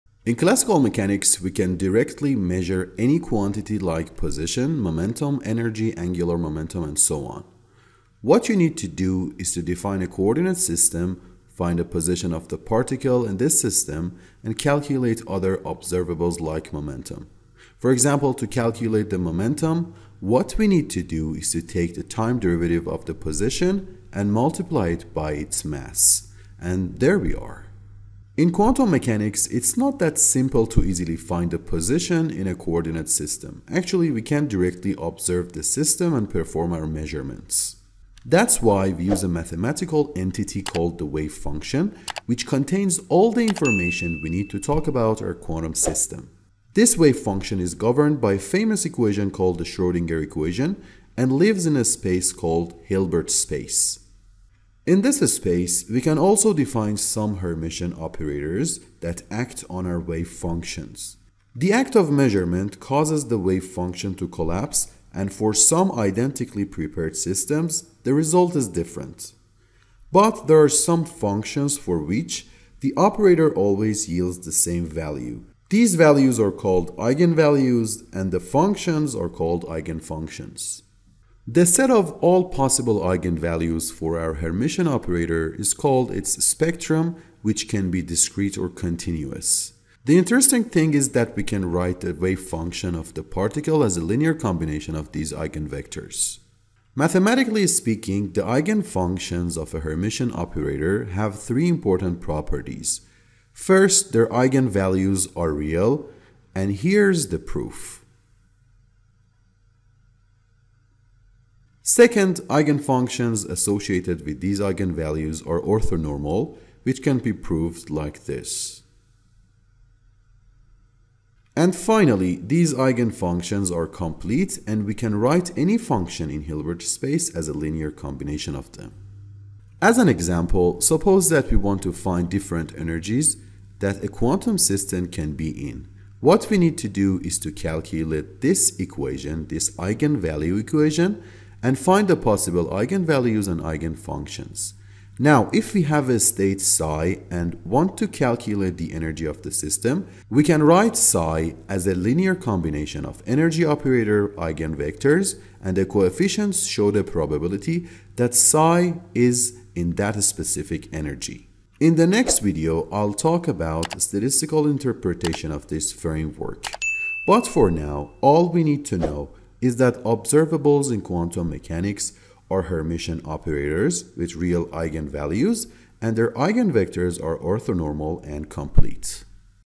Observables in Quantum Mechanics
In this video, I talk about observables in quantum mechanics. Observables are Hermitian operators whose eigenvalues are real and representation of a physical value. These eigenvalues and eigenfunctions live in Hilbert space which is a vector space itself. In the next video I'll talk about statistical interpretation of quantum wavefunctions and we'll see how we can derive physical concepts from this mathematical framework.

In this series of videos I'm going to share some information about quantum physics based on the amazing book "Quantum Mechanics" by David J. Griffiths.